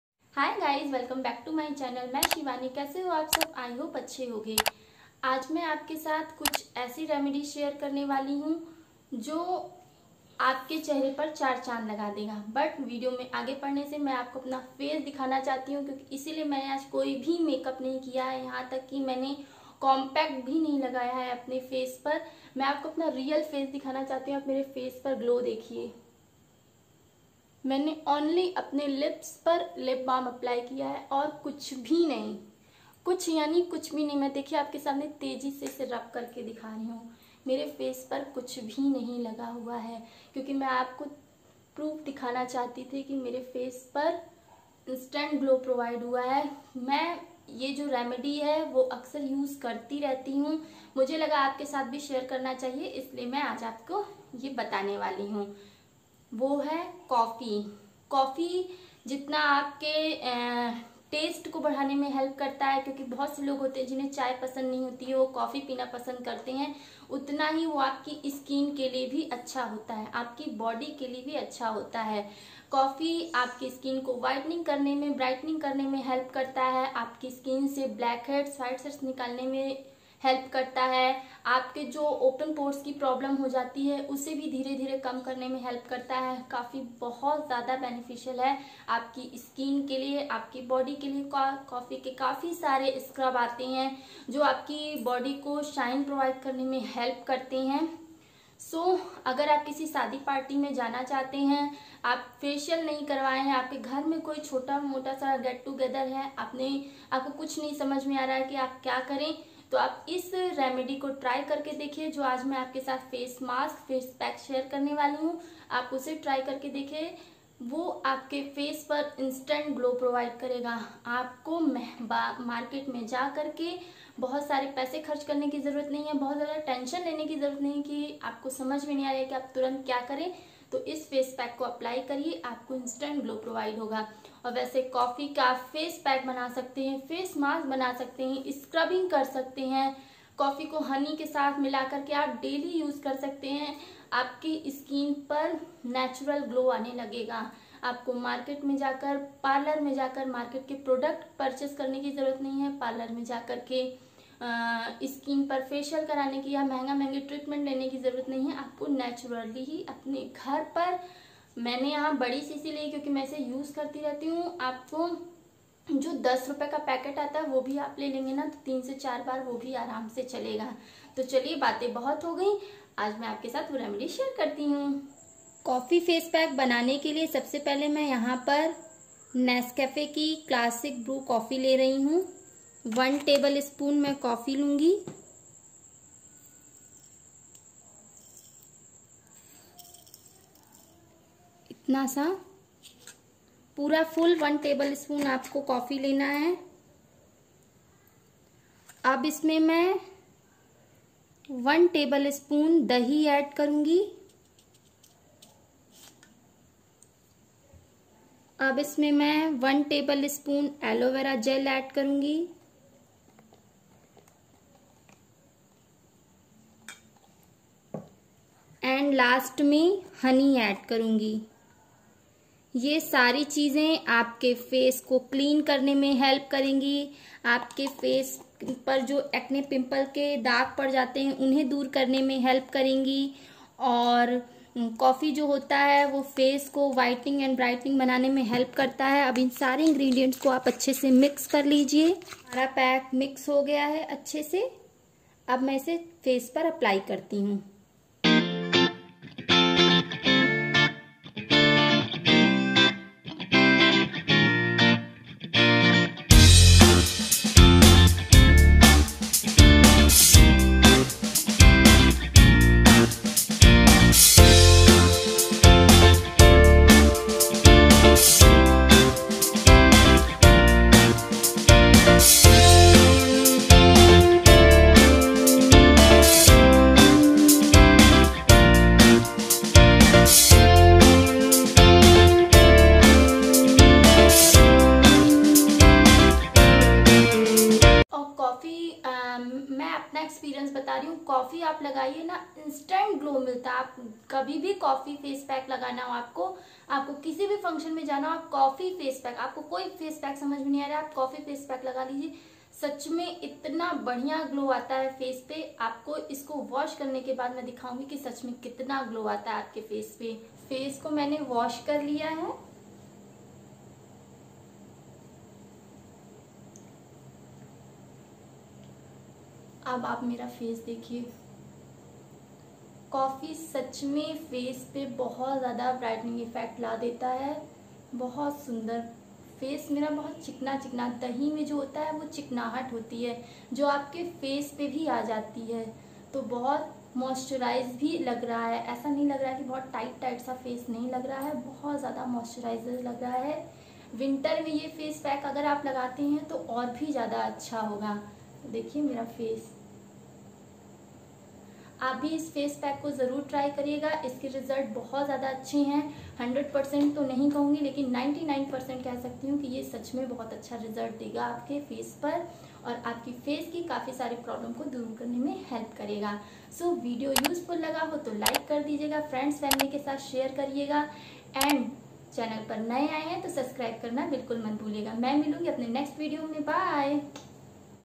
Skin whitening coffee face pack!! Remove dark spots & Pigmentation
Hello friends

coffee facial, कॉफी फेशियल से पाइए गोरी रंगत

सिर्फ एक पैकेट कॉफी चेहरे को इतना ज्यादा गोरा बना देगी कि खुद ही देख कर चौक जाएंगे

स्किन व्हाइटनिंग, skin brightening, anti aging, dark spot, open large pores, skin whitening treatment, anti wrinkles, coffee facial, home facial, coffee scrub

Shivifam